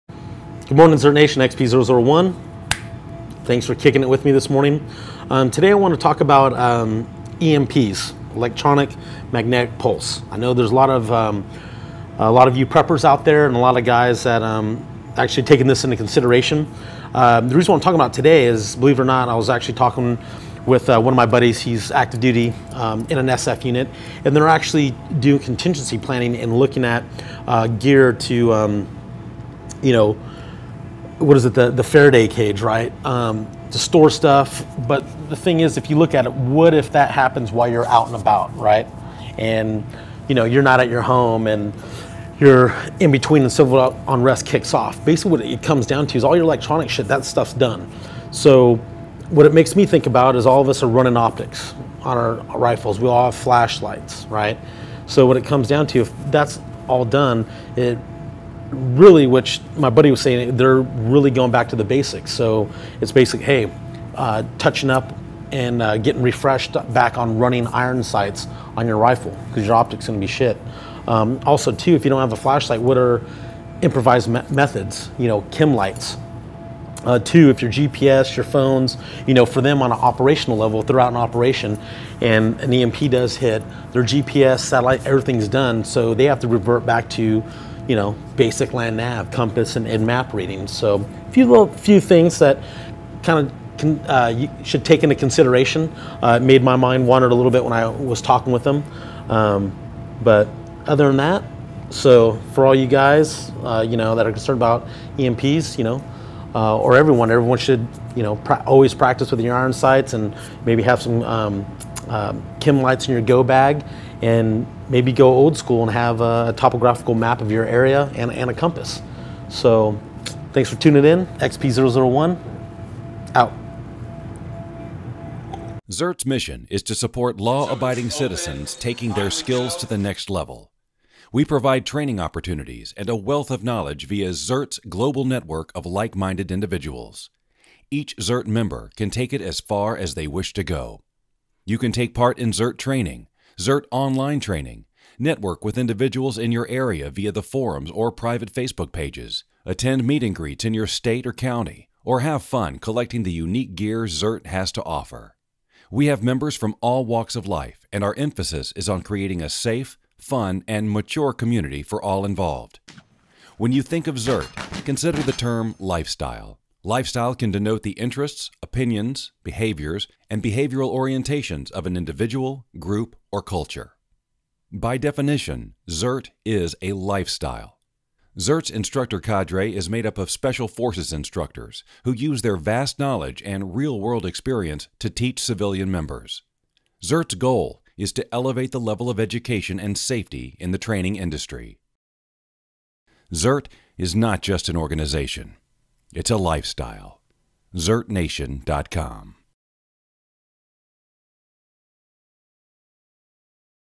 Z.E.R.T.'s Coffee & Hate w/ XP001 Episode 26
Coffee & Hate with XP001. This morning's topic: ELECTROMAGNETIC PULSE -EMP.

P.S. Yep I said electronic - a little too much hate this morning.

PS - Yep, said electronic - too much hate this morning.